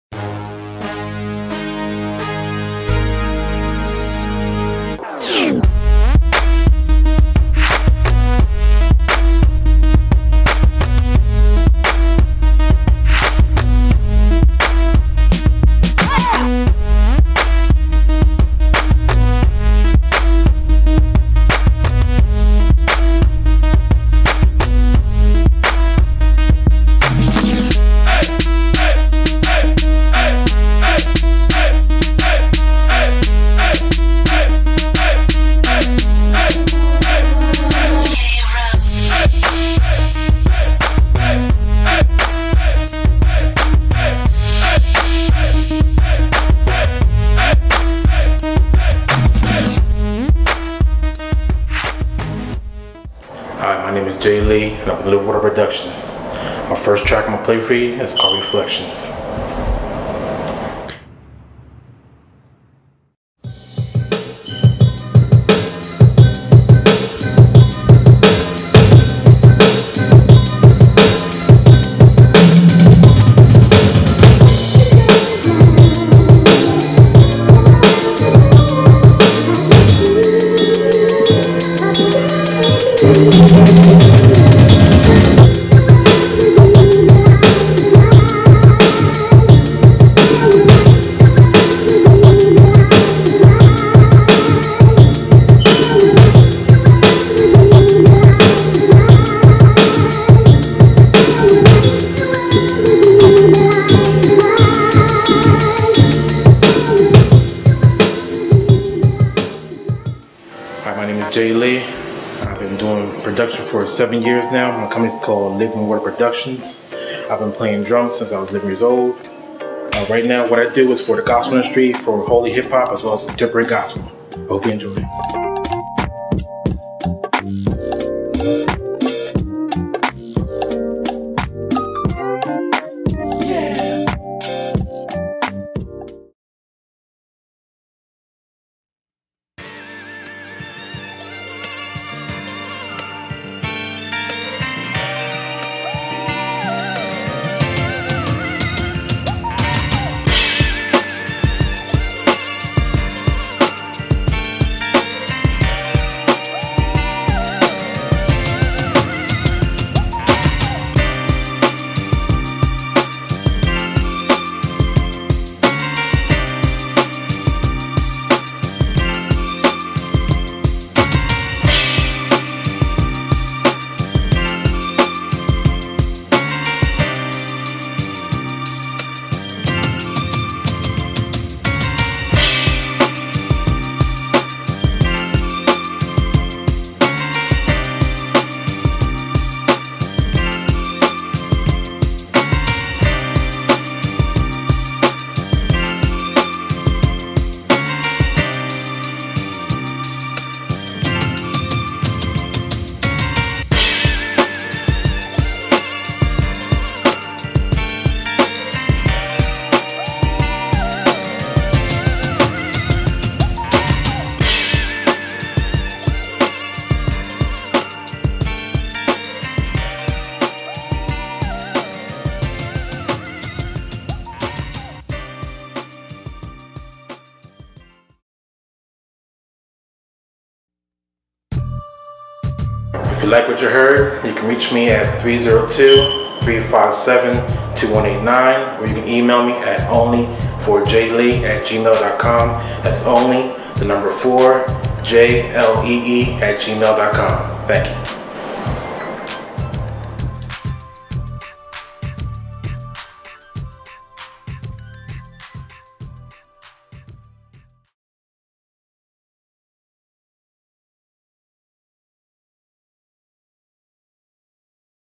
G.A.M.E. Ent. Artists Spotlight ft. Jay Lee (Producer) IMAX-HD
Jae Lee is the owner of Livin' Water Productions which was founded in 2005. He currently lives in Wilmington, Delaware with his wife and three daughters. At the early age of 11, he played drums and developed his love for composing music. He currently specialize in Contemporary Gospel and Spiritual Hip-Hop.